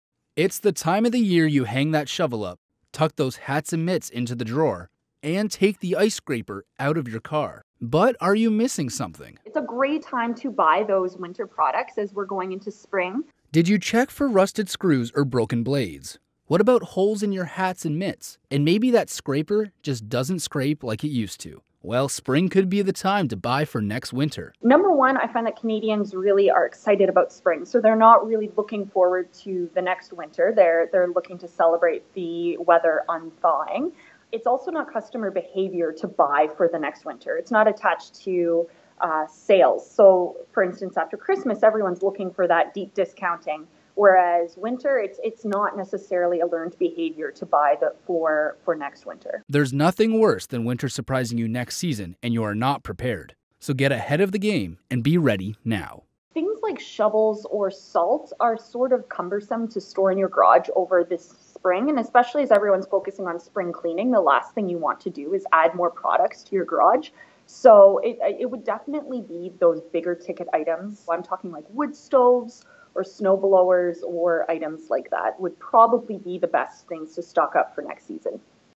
FOR SALE: The MAJOR (winter) mistake you're making every spring
Planning ahead and getting some sweet deals is something you should be putting top of mind at the end of every winter season. ---